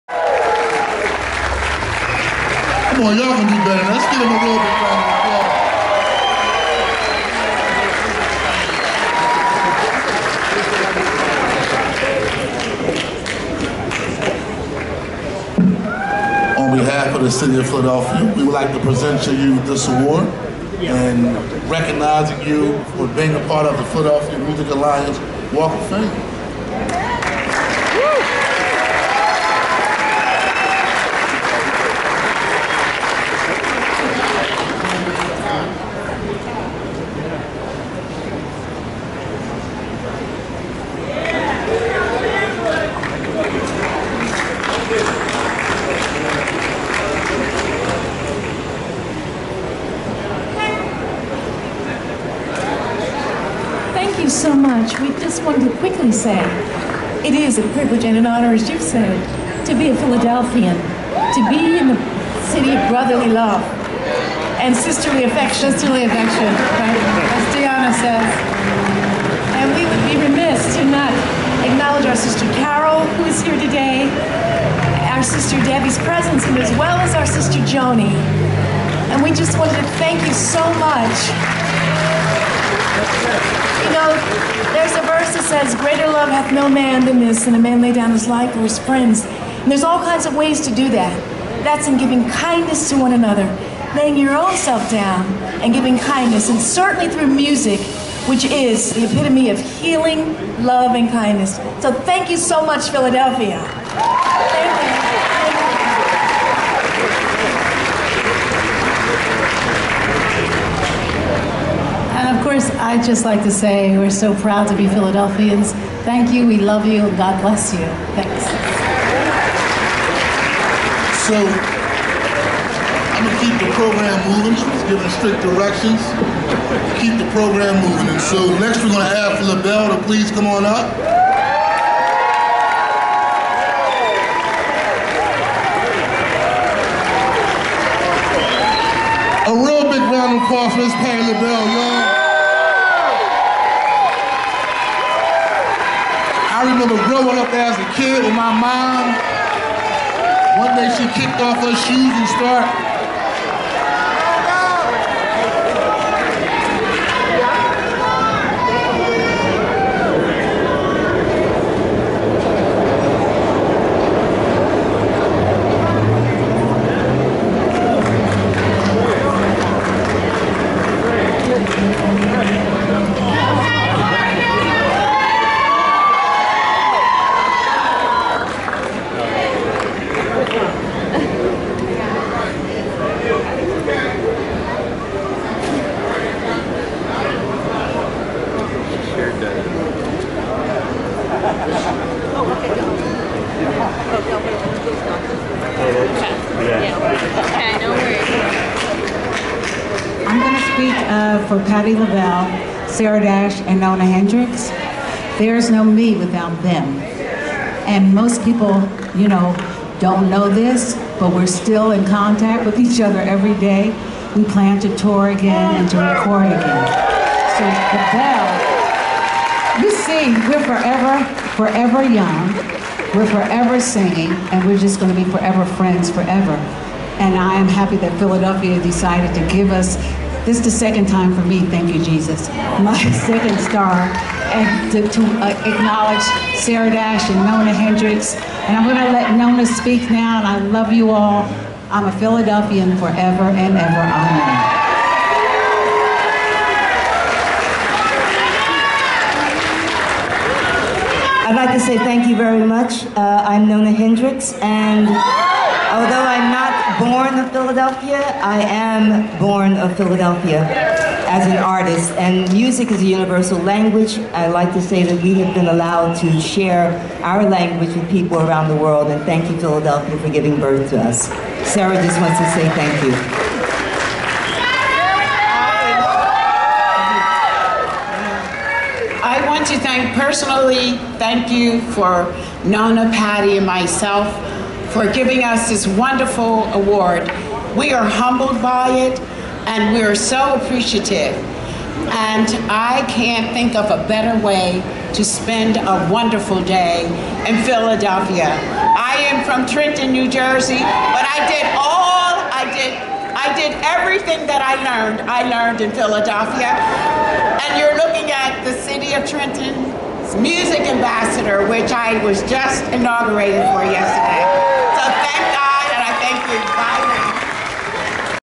Philadelphia Music Alliance’s 2017 Walk Of Fame Honorees
Patti LaBelle Nona Hendryx & Sarah Dash